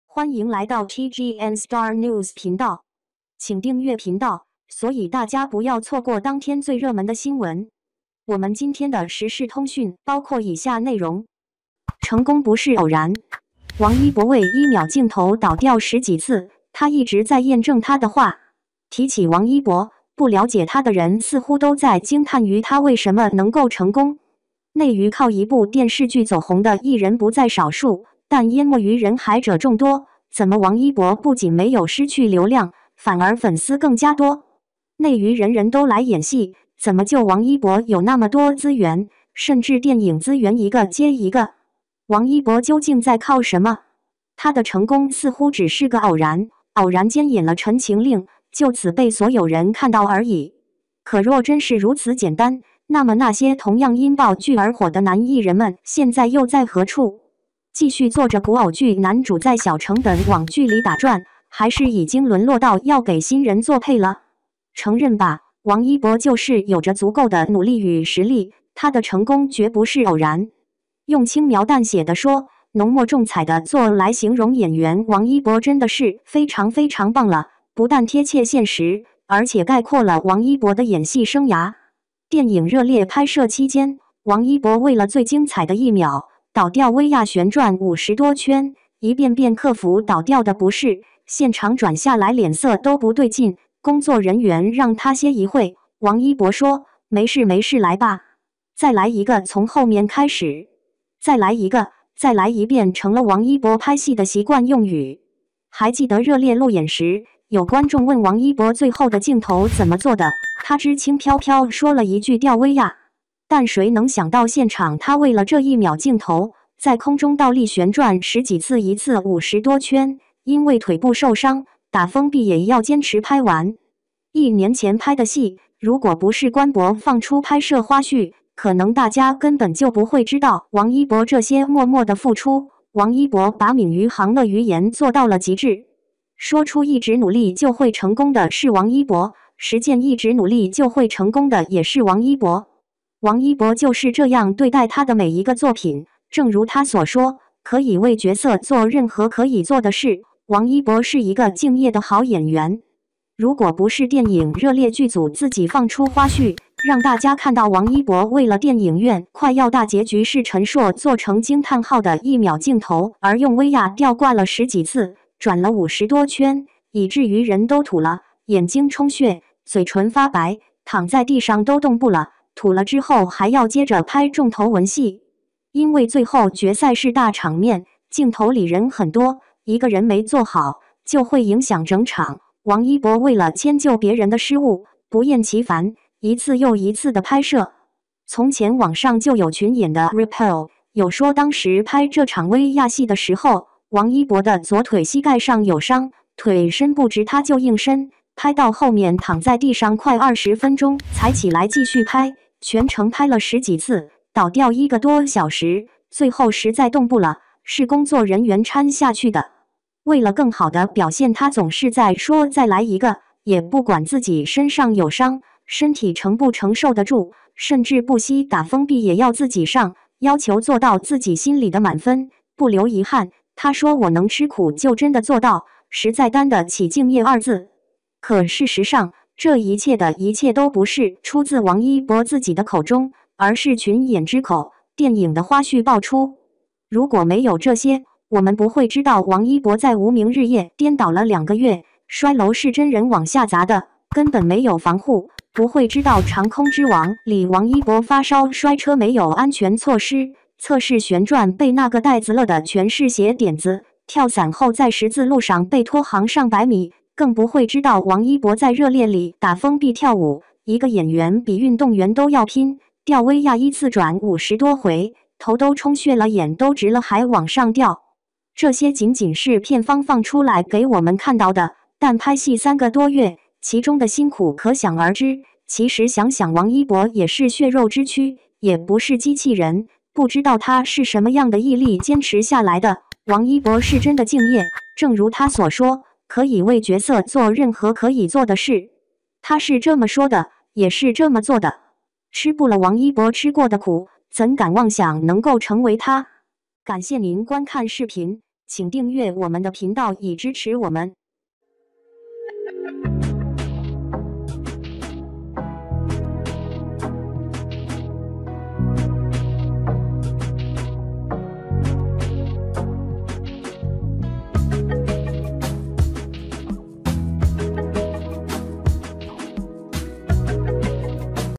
成功不是偶然！王一博为1秒镜头倒吊十几次，他一直在验证他的话！
成功不是偶然！王一博为1秒镜头倒吊十几次，他一直在验证他的话！| TGN Star News